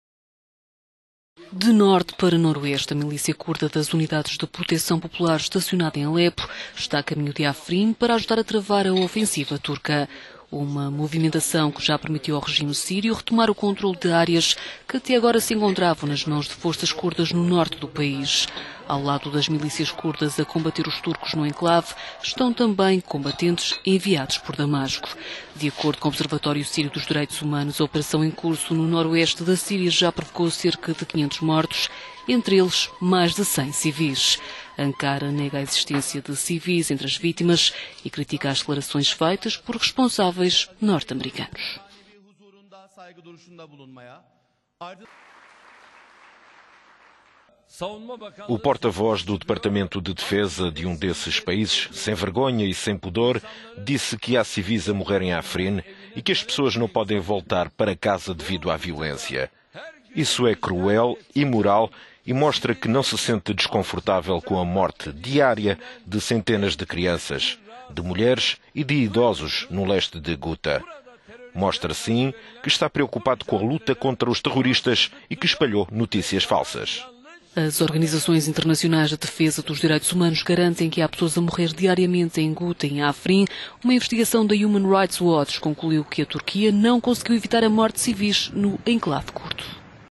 Síria: Curdos reforçam posições em Afrin
A operação em curso há pouco mais um mês no enclave de Afrin, no noroeste da Síria,  já provocou cerca de 500 mortos. Mais de 100 são civis…